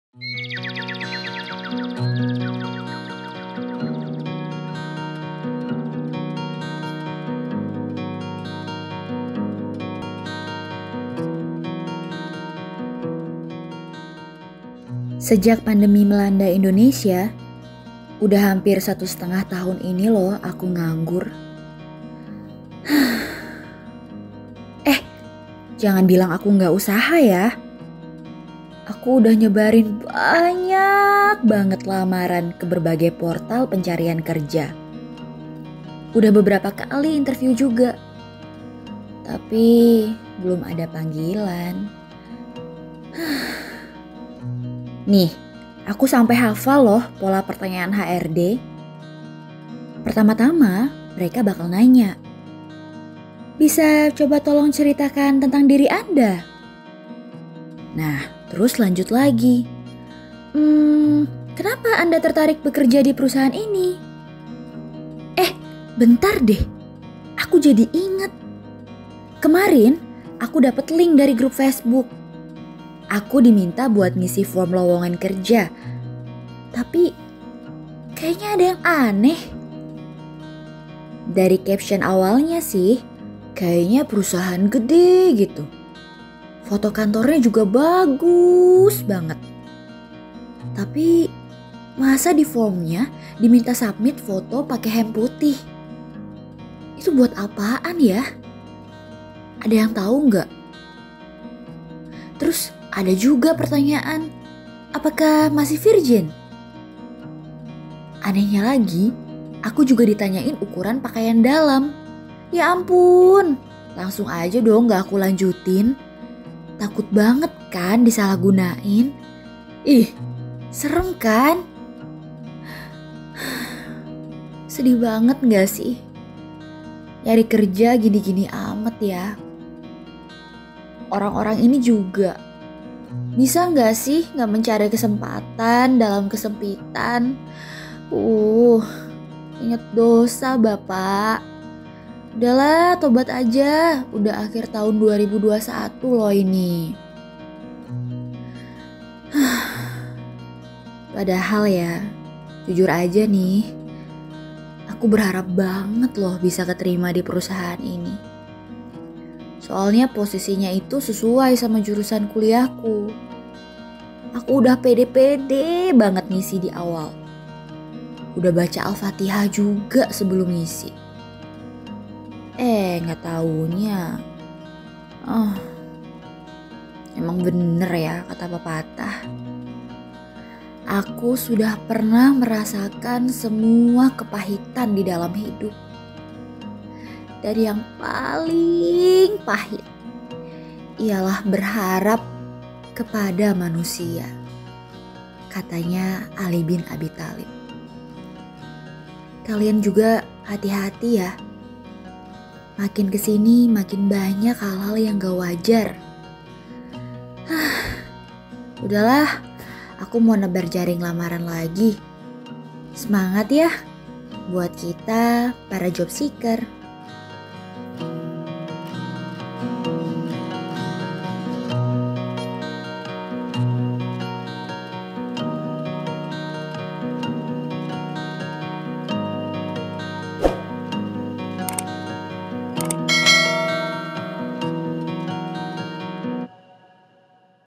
Pertanyaan Aneh di Form Lowongan Kerja
Kalau kalian, pertanyaan teraneh di form lowongan kerja yang pernah kalian dapat apa nih, SoHip?

Yuk SHARE dan kasih LIKE ??
Nantikan video terbaru dan inspiratif lainnya dari Hipwee di sini.